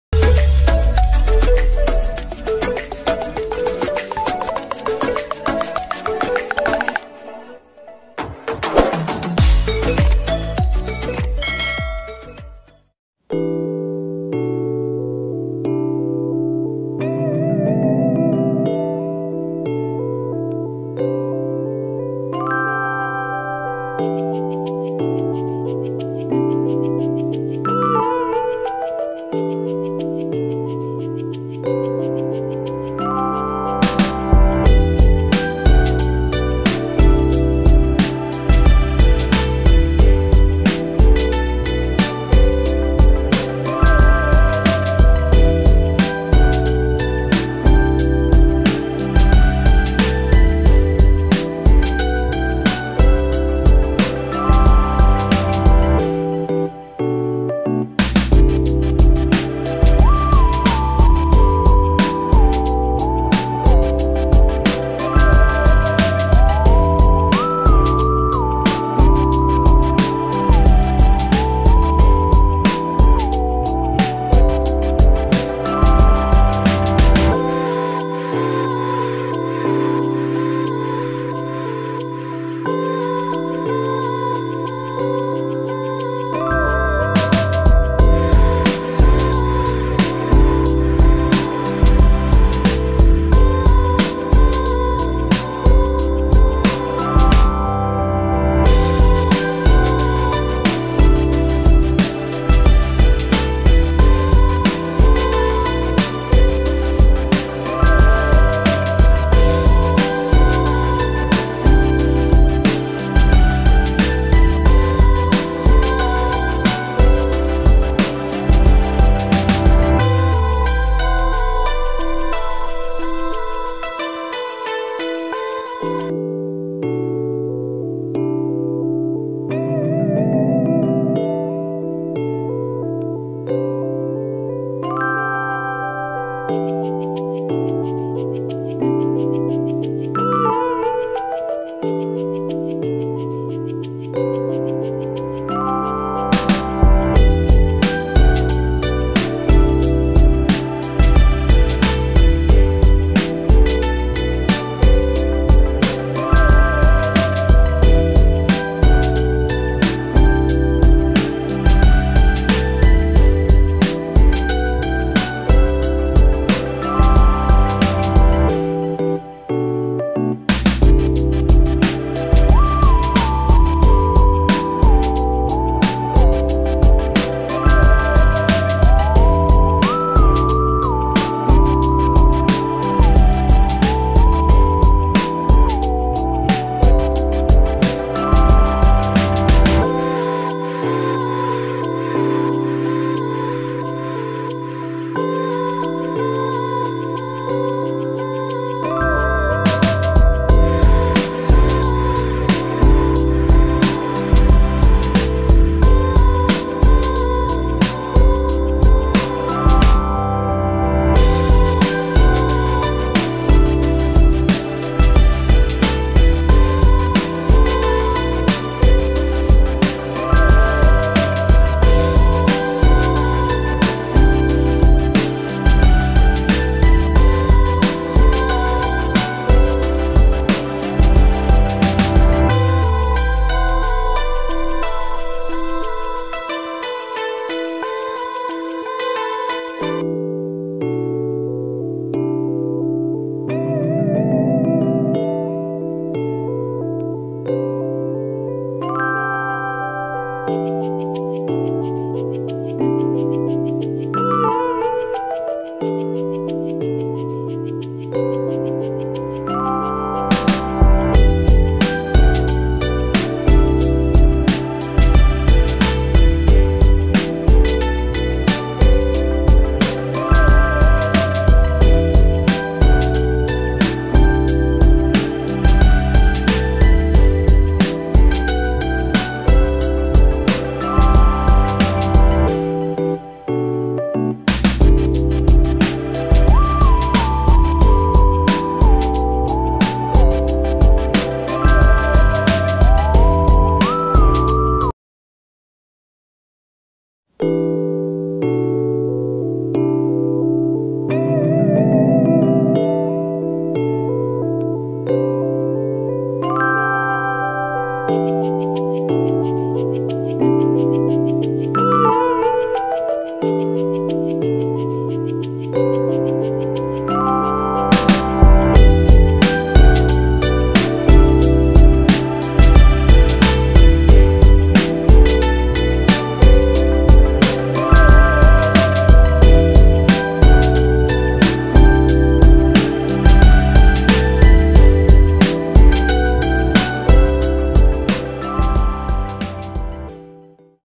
MORNING ROUTINE || RUTINITAS PAGI2 SALAH SATUNYA YAA MASAK || MASAK JENGKOL + TELOR BALADO
Kembali lg di vlog sya

nah dividio kali ini sya mau masak2 masakan sederhana ala2 sya ,

selamat menonton